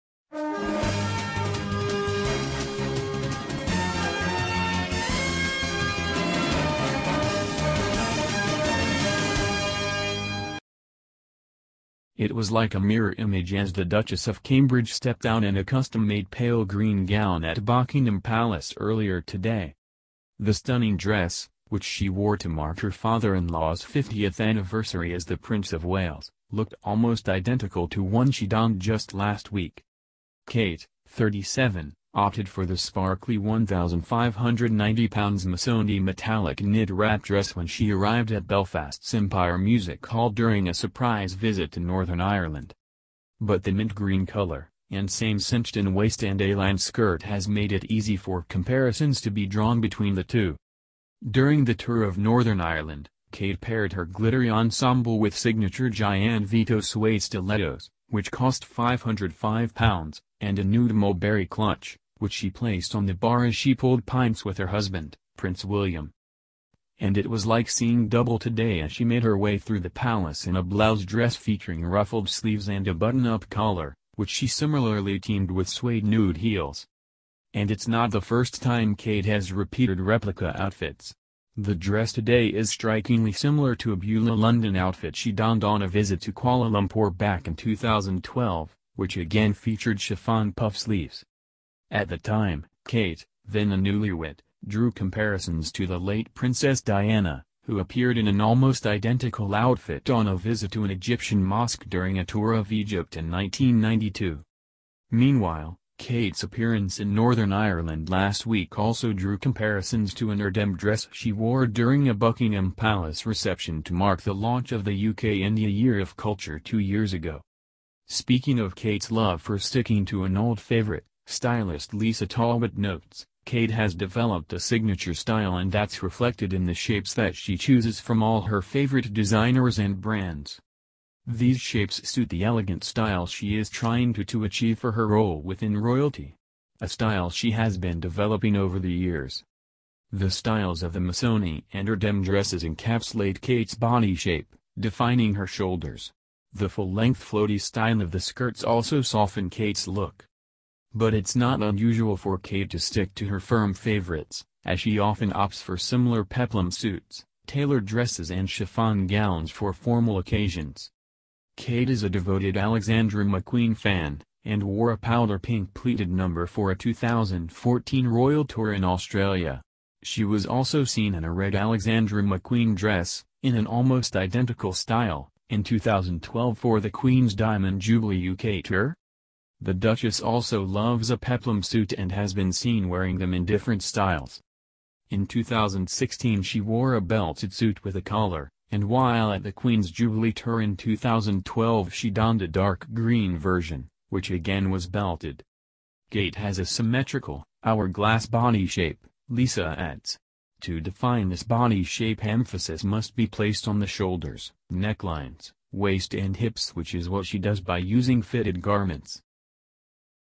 Seeing double! Kate Middleton's dress bears a very striking resemblance to her outfit last week
Seeing double! Duchess of Cambridge's mint custom-made dress bears a VERY striking resemblance to the £1,590 Missoni gown she debuted in Belfast last week